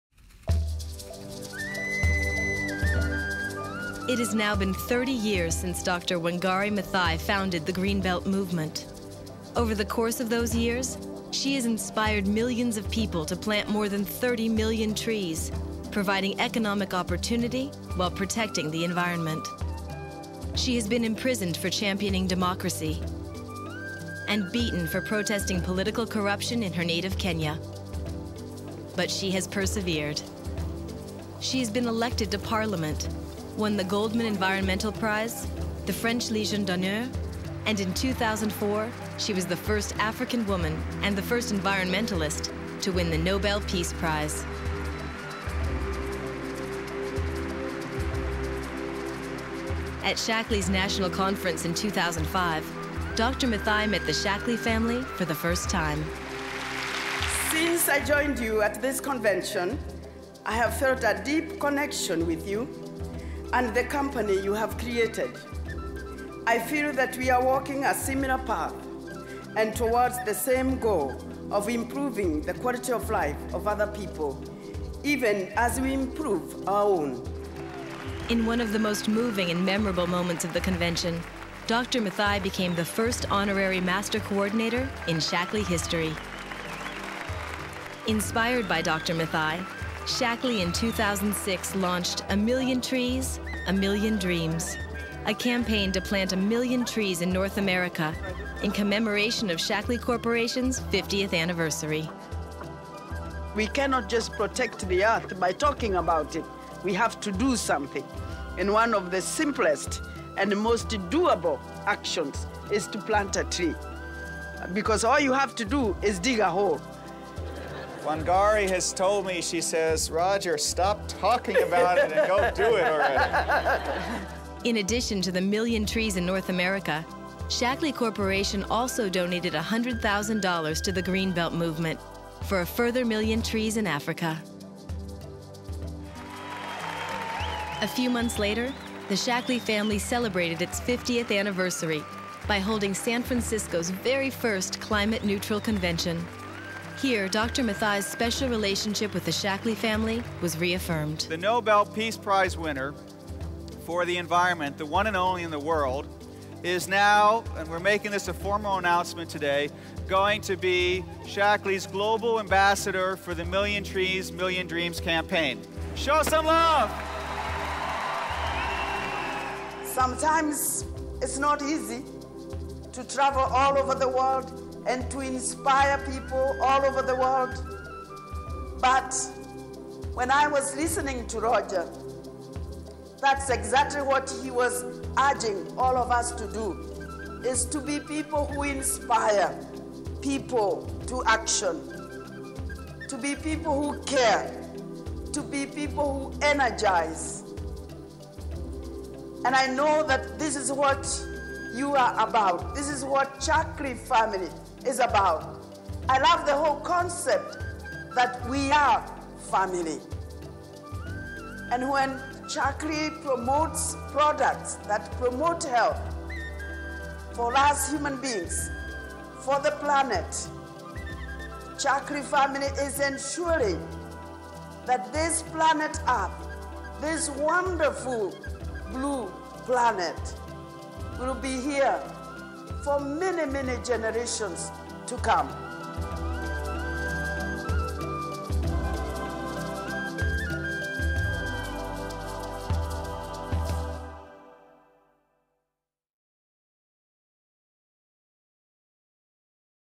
Prof. Wangari Maathai 1940-2011
Today we remember Prof. Wangari Maathai, educator, environmentalist, Nobel Laureate and a friend of us all.